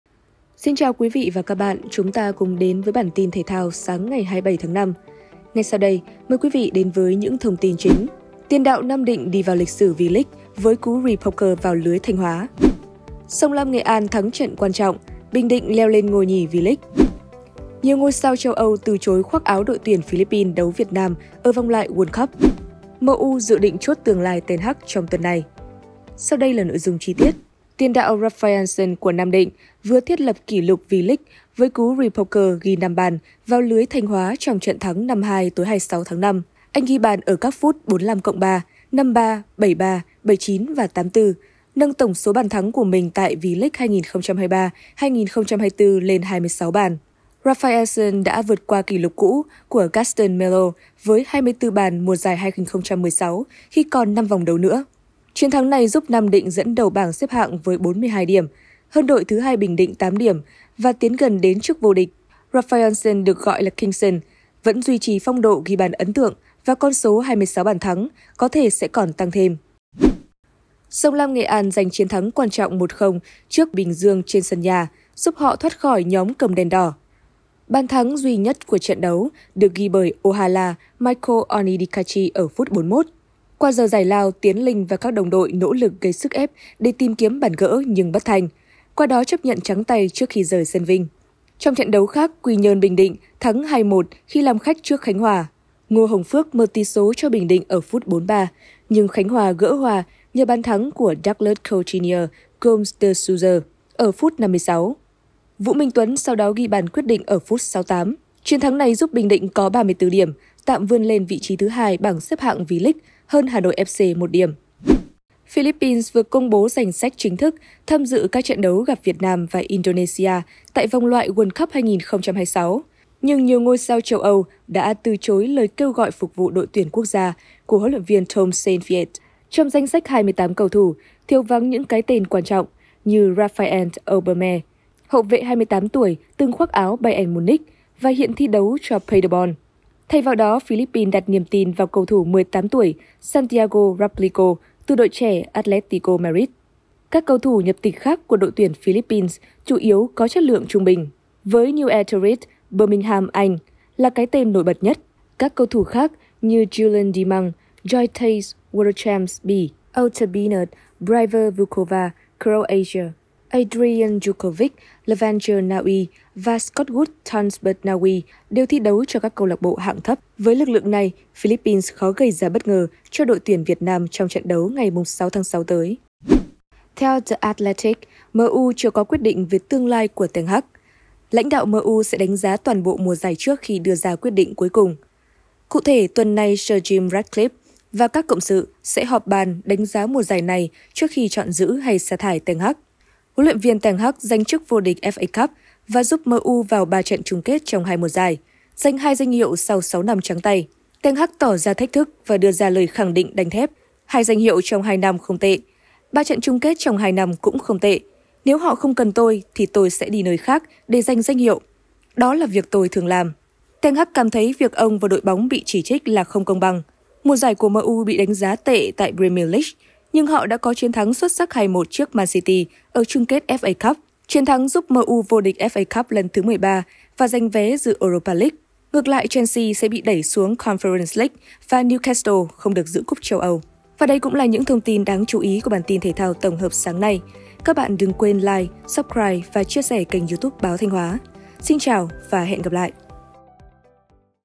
Tin thể thao 27/5: Tiền đạo Nam Định đi vào lịch sử V-League với cú repoker vào lưới Thanh Hoá
00:00 Mời quý vị đến với những thông tin chính:
00:27 Tiền đạo Nam Định đi vào lịch sử V-League với cú repoker vào lưới Thanh Hoá
01:10  SLNA thắng trận quan trọng, Bình Định leo lên ngôi nhì V-League
01:55 Nhiều ngôi sao châu Âu từ chối khoác áo ĐT Philippines đấu Việt Nam ở vòng loại World Cup
02:56 MU dự định chốt tương lai Ten Hag trong tuần này